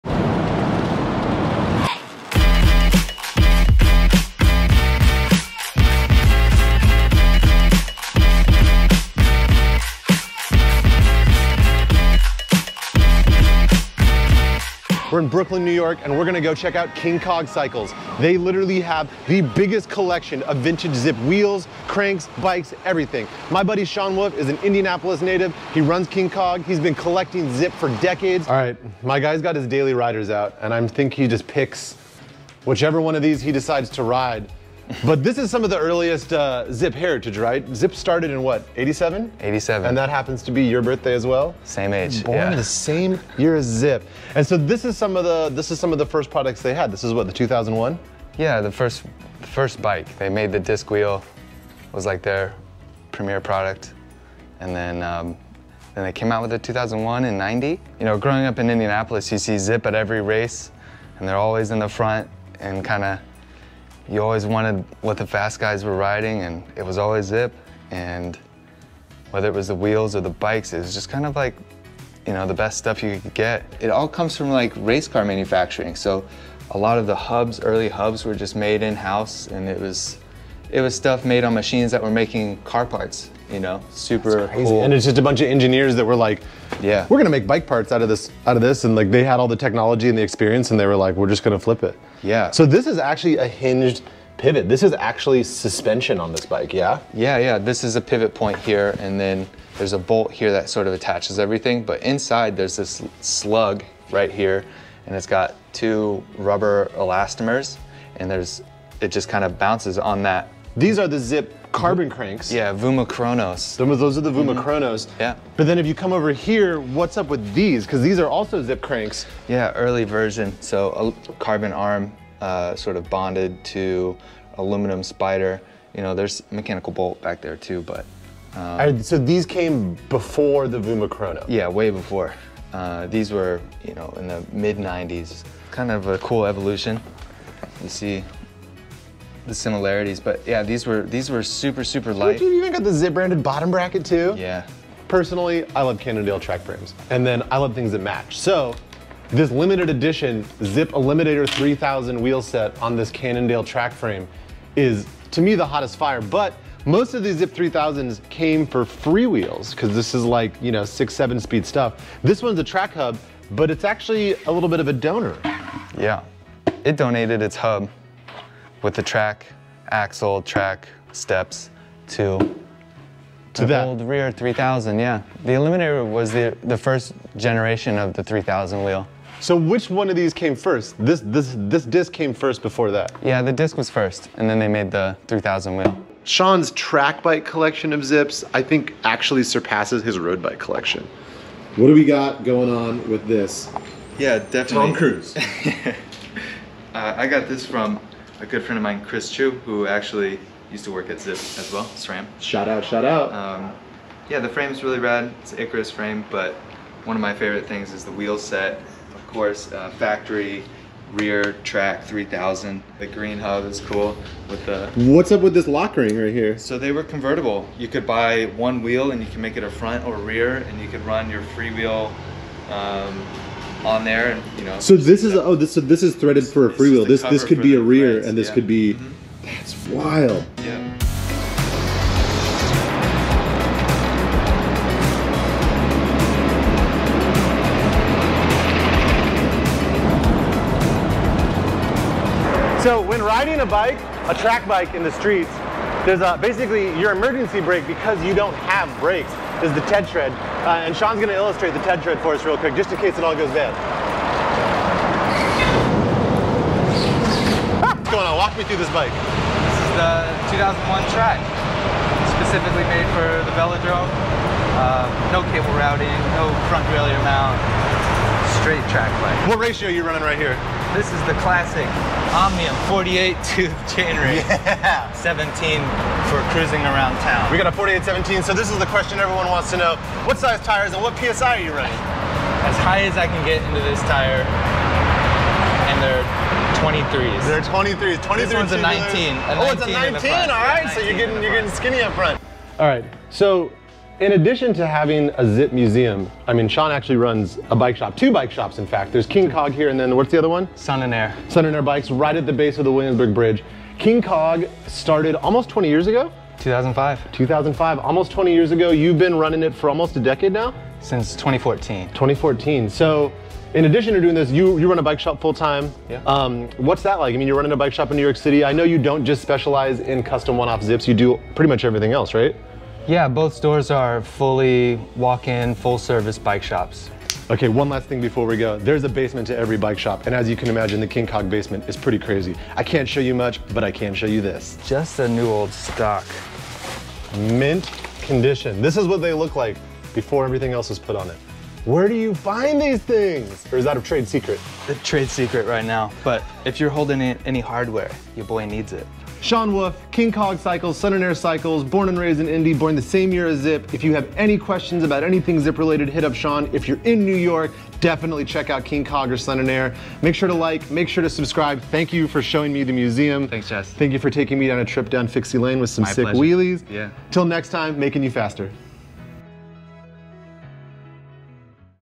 King Kog: Back on Track Bikes
Celebrate 1 YEAR of Making You Faster with us this week as we step inside the LEGENDARY Brooklyn bike shop, King Kog. Owner and Indianapolis native Shawn Wolf has been collecting all things Zipp for so long he has a veritable museum of Zipp history hanging throughout the shop. Everything from ultra-limited edition 2001 track bikes to almost every generation of Zipp wheel can be found inside. Take a walk through history as we celebrate our future on this week’s Making You Faster.